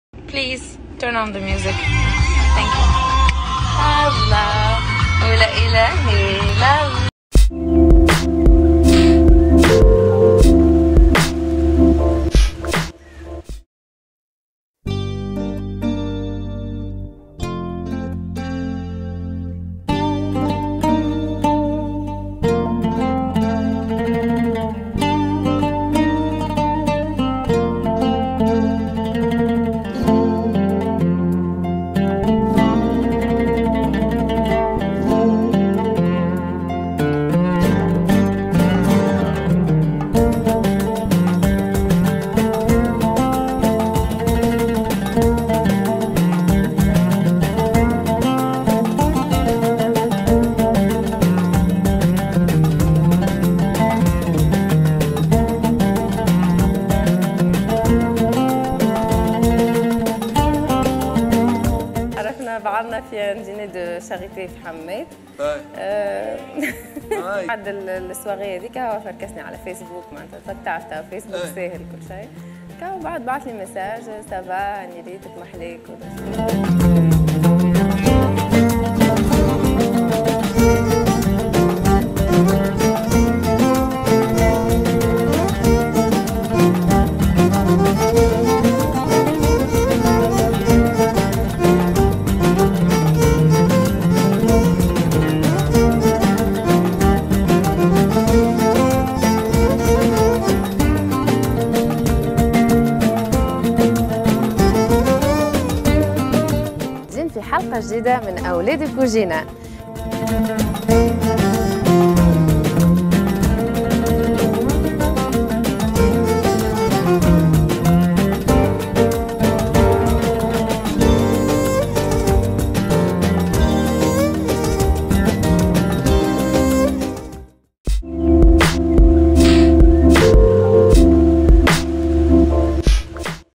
Celebrity - Qui est Amira Jaziri ? أميرة الجزيري ⚡
Portrait de Amira Jaziri , l'actrice et la femme de Youssef Msakni .
Qui est Amira Jaziri ? Découvrez la page Wiki de Amira Jaziri avec sa biographie, Age, Mariage, Tailles et plus encore